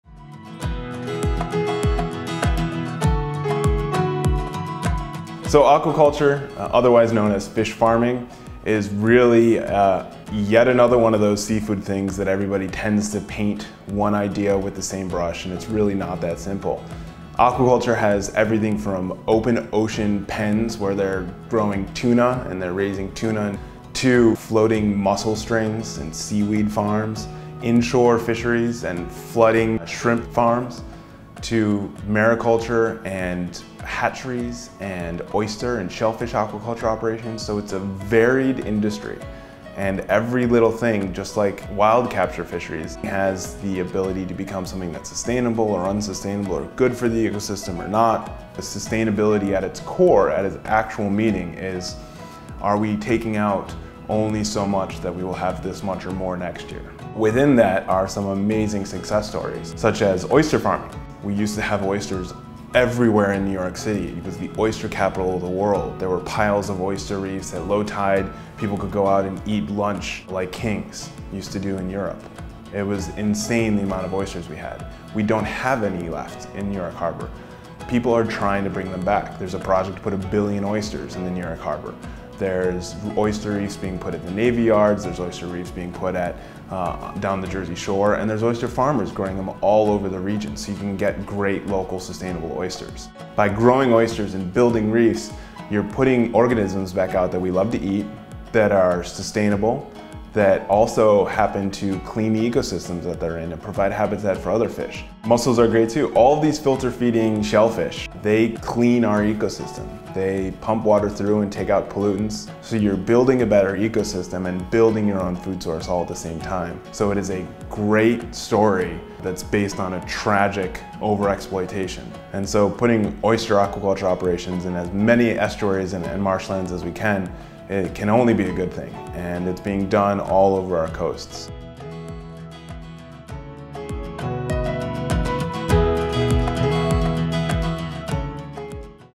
Aquaculture and the Amazing Oyster
Learn all about aquaculture and why oysters are so amazing! In this video, Sean Dixon also gives great tips on how to enjoy seafood sustainably, and sheds light on the history of New York City and the role oysters have played.